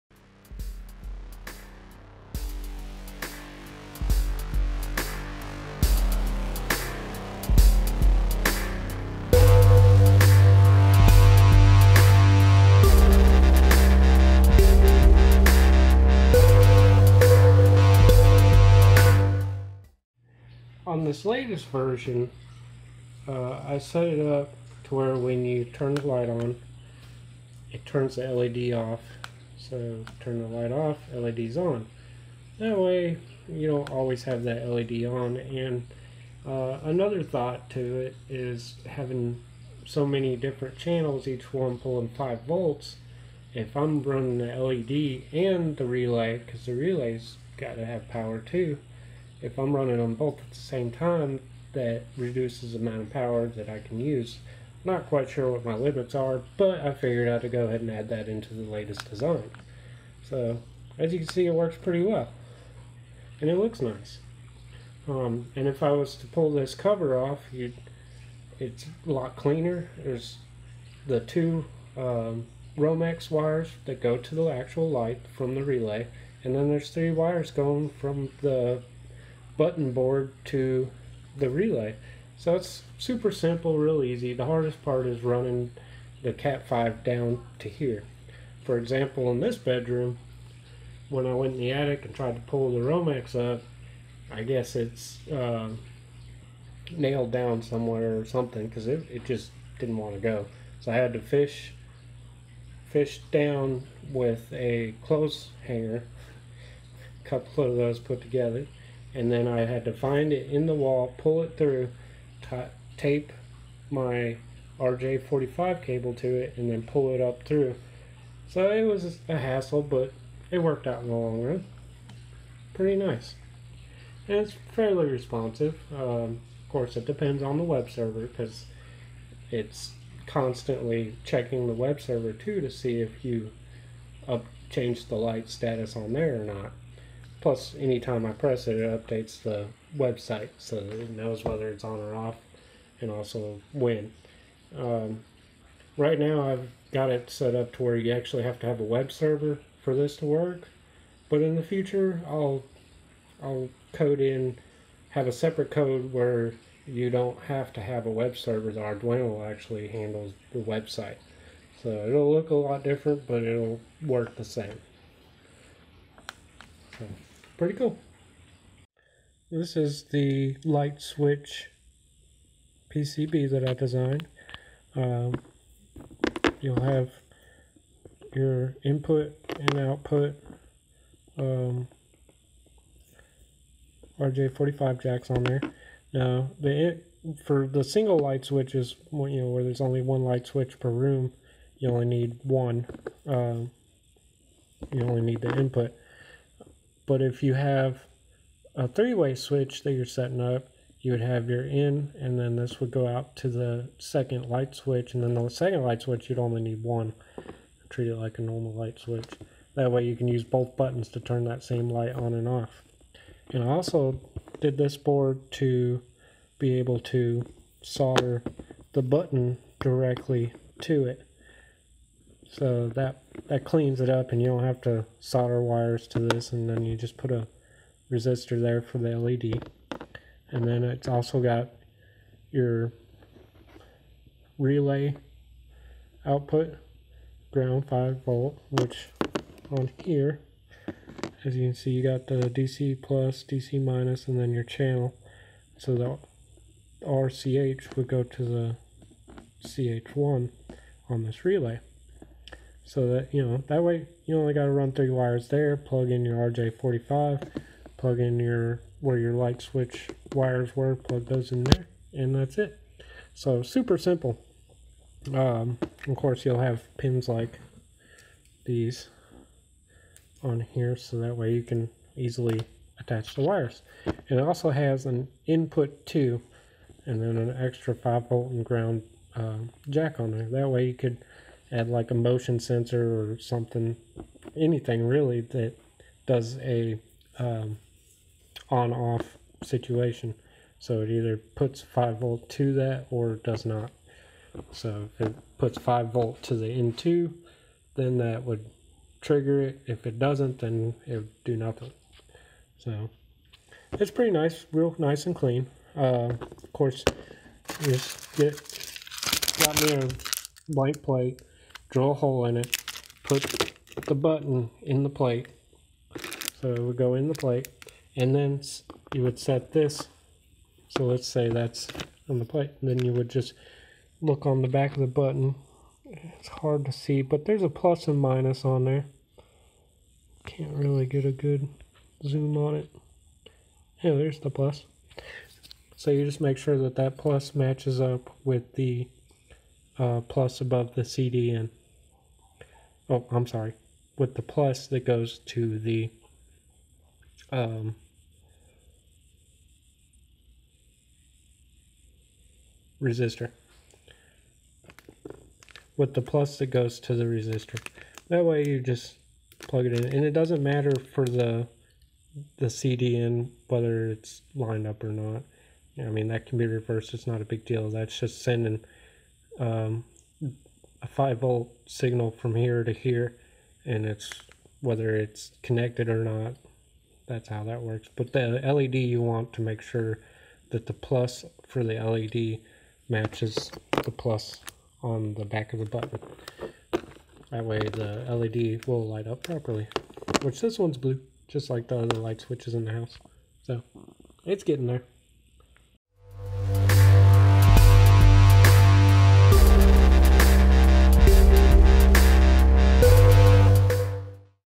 My Arduino Home Automation System - Light Switch Buttons
My Arduino Home Automation System

This video explains how the light switch buttons and relays work with the MAH system.

Hardware used for this application:
My Arduino Home Automation Control Board v2.0
LED Light Switch Button
SSR
Arduino UNO
Arduino Eithernet Shield
74HC595 Shift Register (out)
CD4021B Shift Register (in)

Usage Goals:
Control lights in house.
Use buttons as light switches.
Use website as central control.